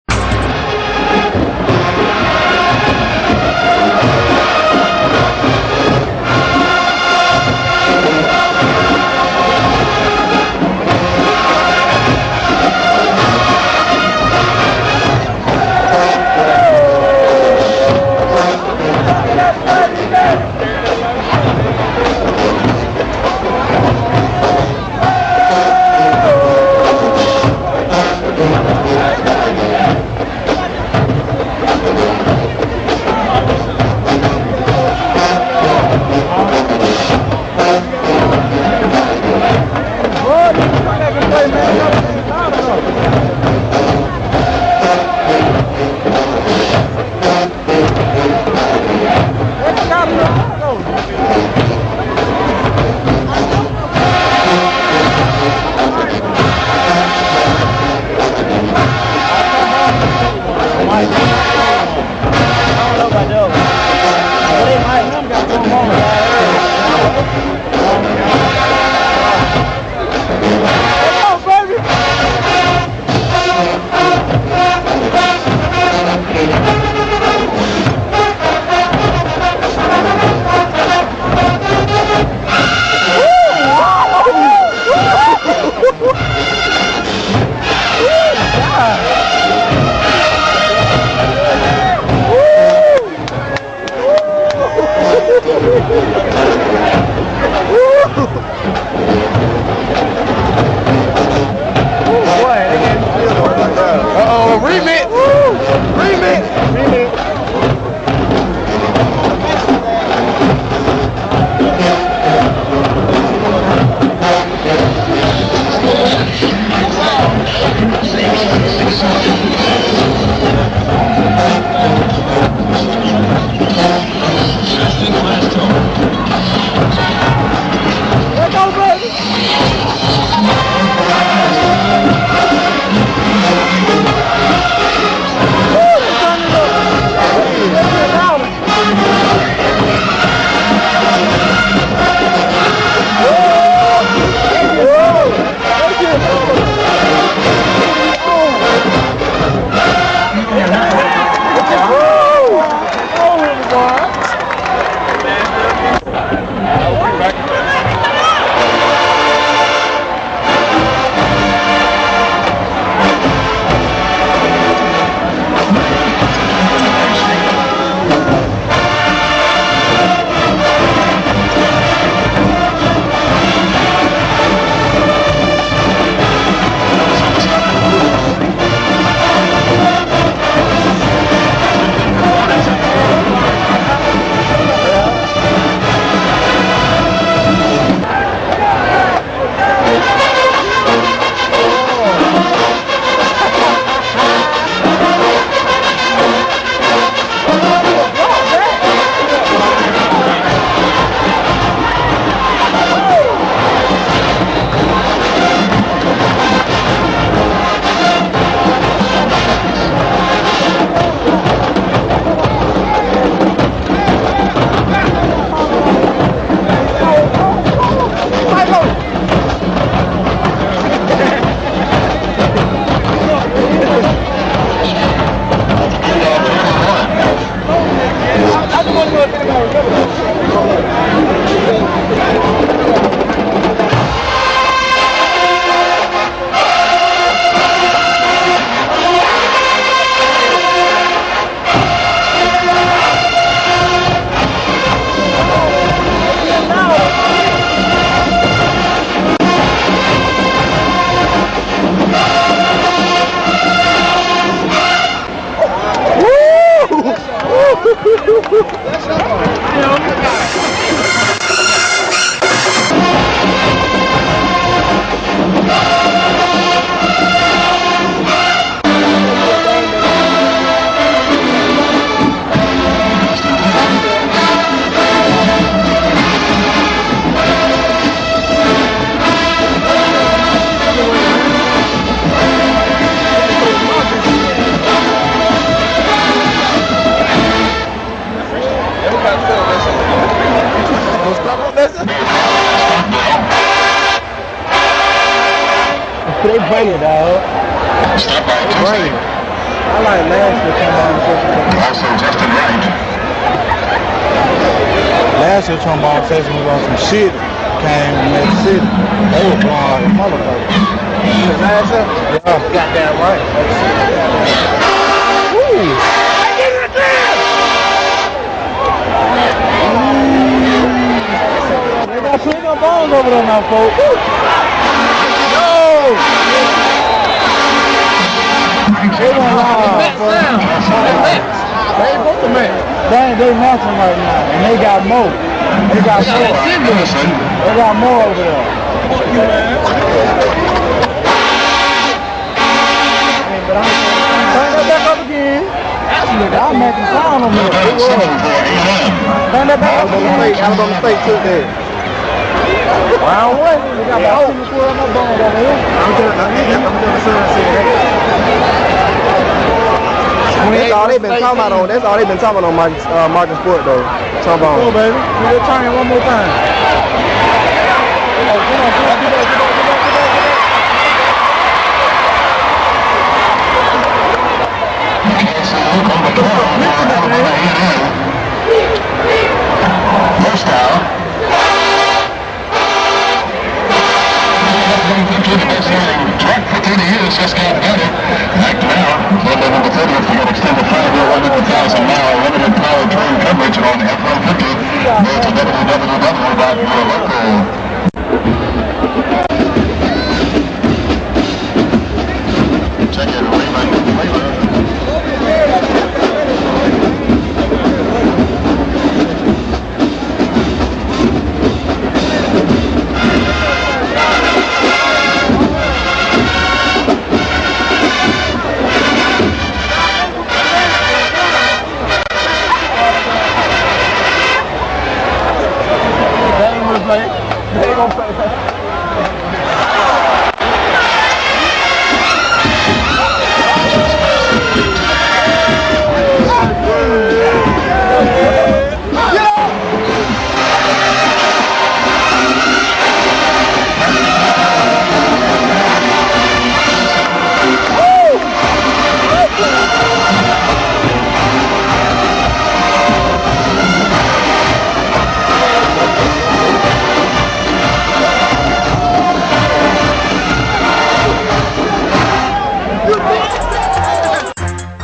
ASU and AAMU-MCC(Magic City Classic)2007
aamu and asu goin at it at the classic in 07..stop by an comment on who u thank was the better band that day